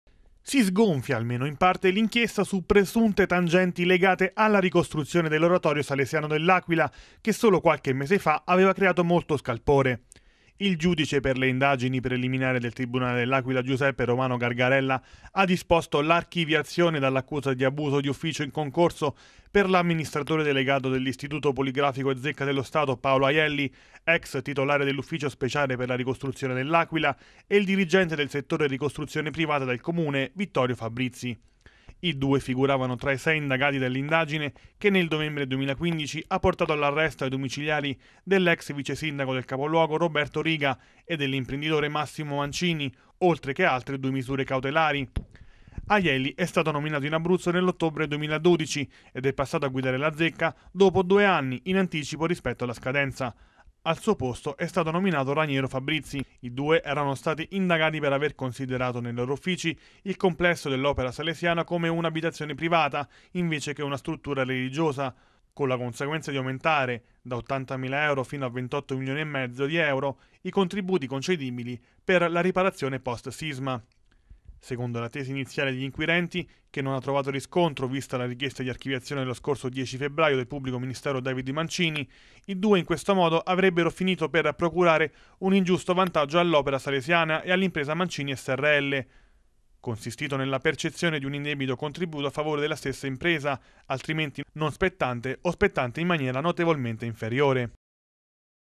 Ricostruzione oratorio e mazzette: inchiesta si sgonfia
Il gip dell'Aquila ha archiviato la posizione di due funzionari pubblici indagati nell'ambito dell'inchiesta su presunte mazzette per la ricostruzione dell'oratorio Don Bosco all'Aquila